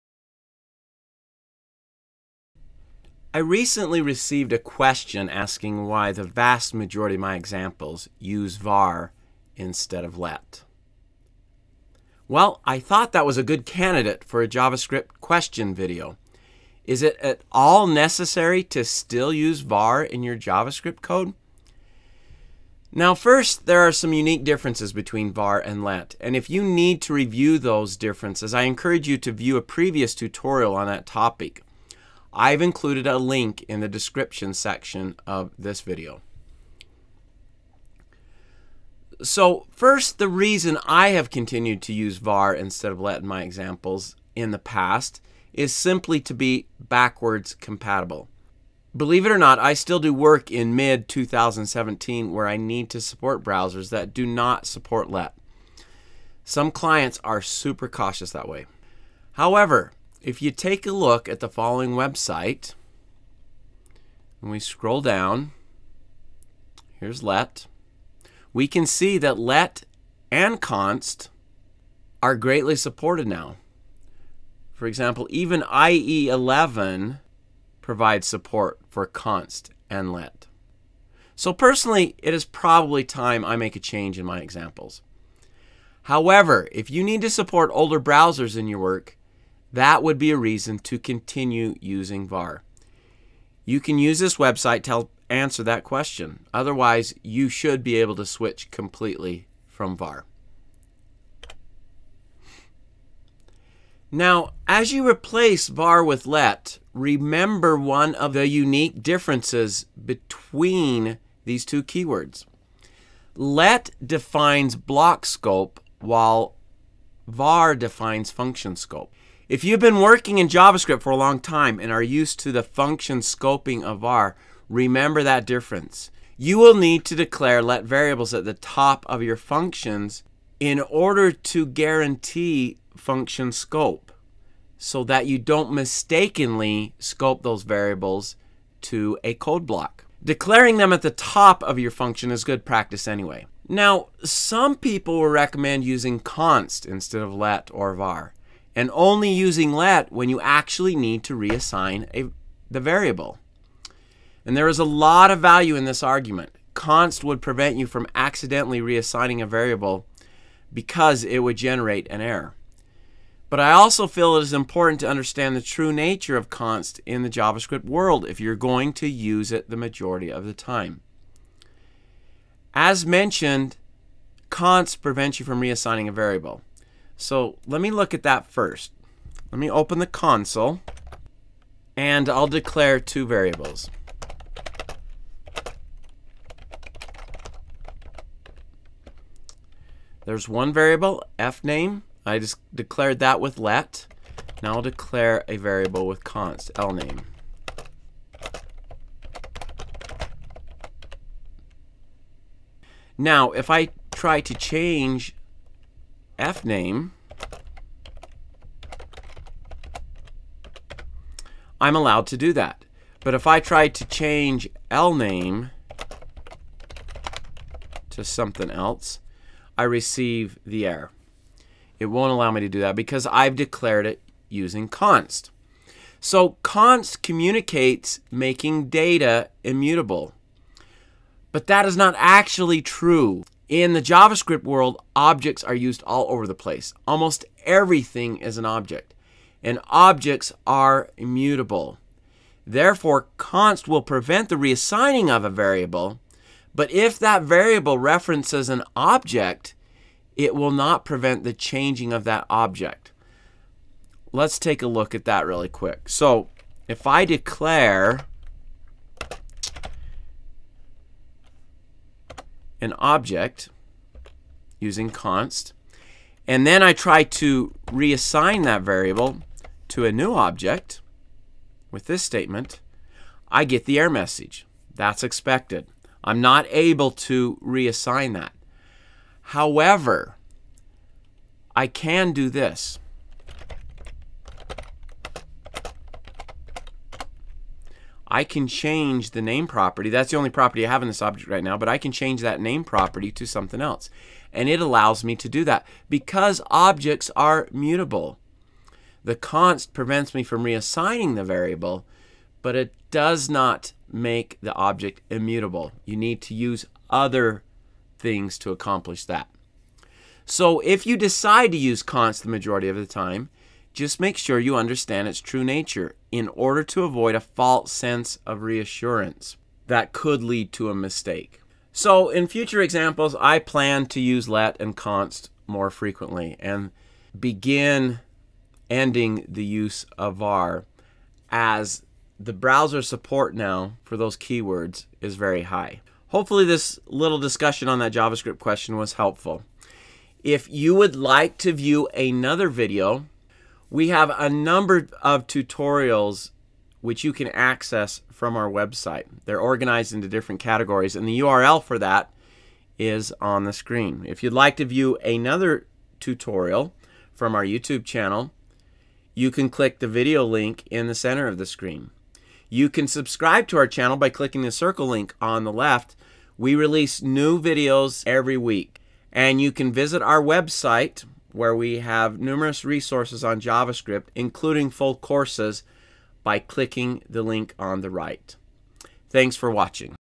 JavaScript Question: Should I Stop Using var and Replace it with let?
let and const are now widely supported by browsers. In this tutorial we address the question of whether there is a need to use var at all. We also address the question of using const for most declarations.

Take the Learn Modern JavaScript: Getting Started or the Learn Modern JavaScript: Advanced Topics courses at a huge discount: